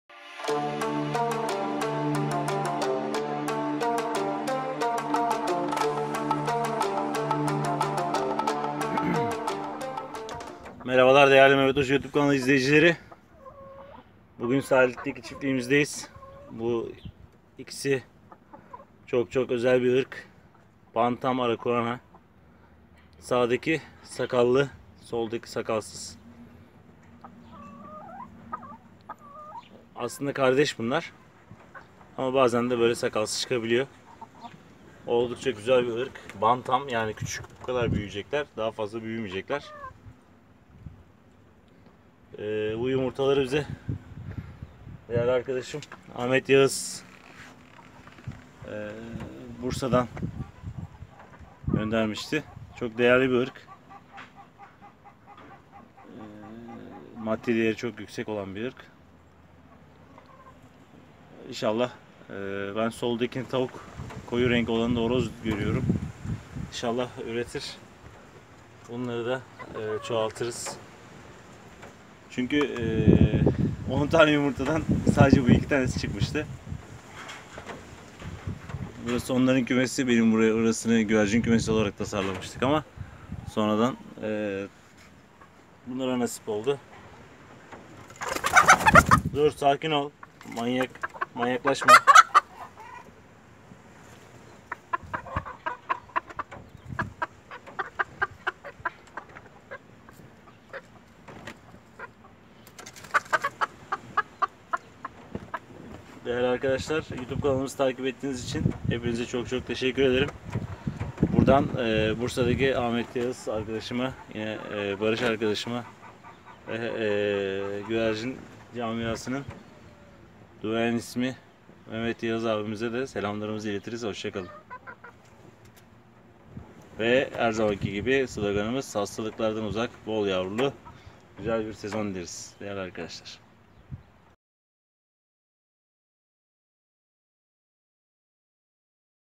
Mavi Yumurtacı Nadir ve Çok Değerli Tavuk Irkı Bantam Araucana
Çok nadir bir ırk olan bantam aracuana ırkı tavuk ve horozumuzu sizler için tanıttık.